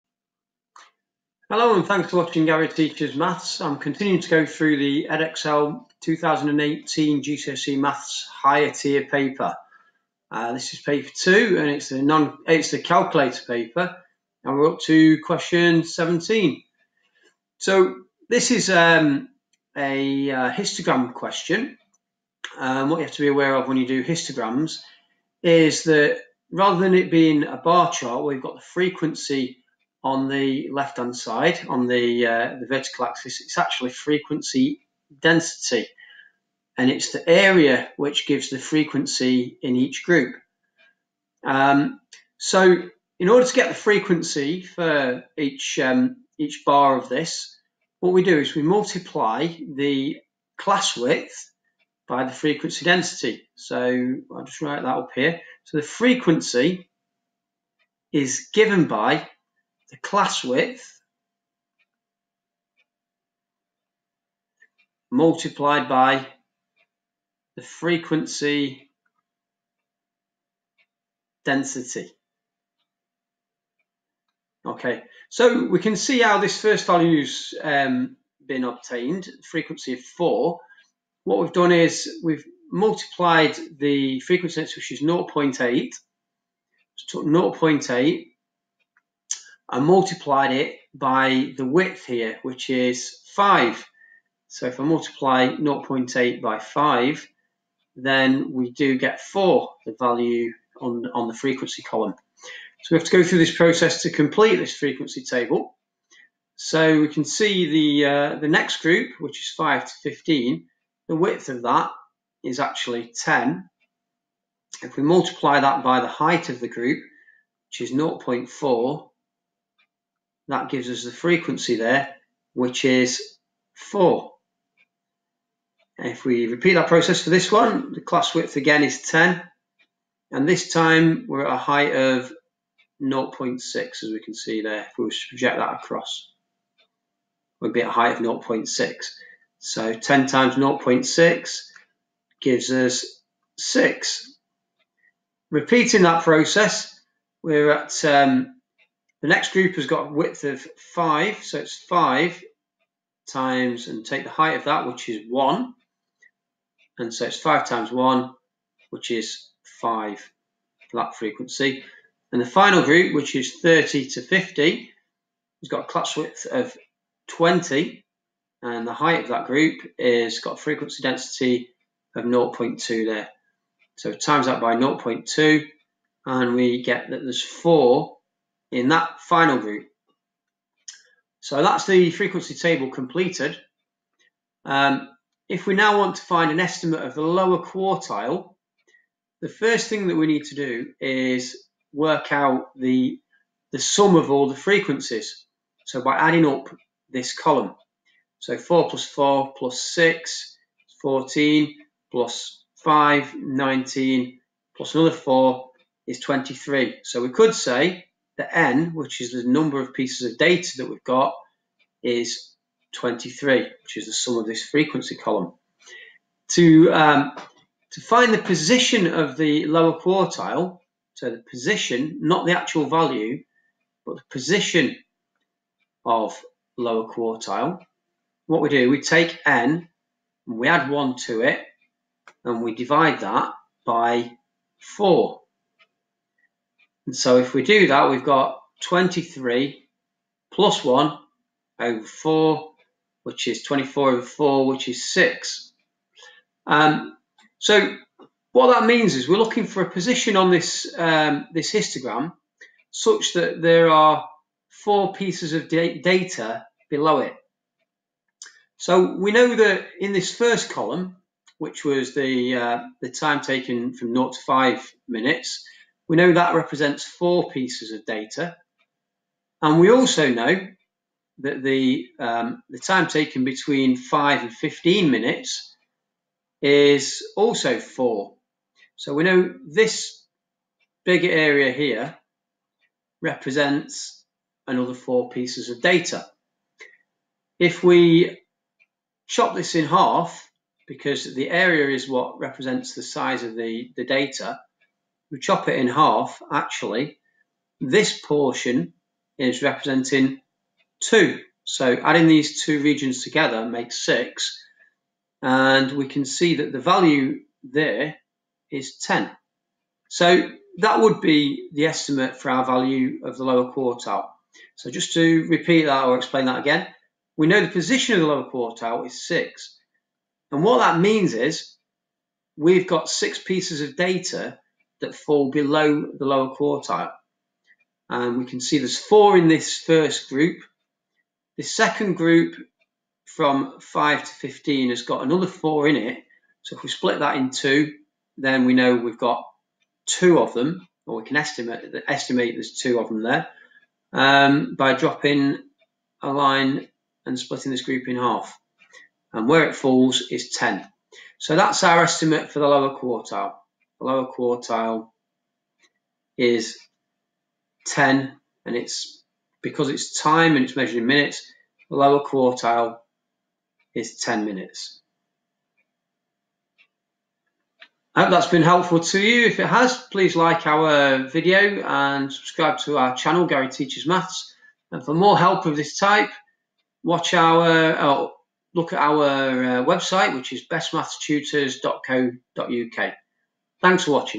17 Histograms, Edexcel 2018 Maths Gcse Higher Tier Paper 2 Question 17
This is question 17 and is about histograms. Here we are asked to fill in a frequency table from a histogram, and then estimate the lower quartile.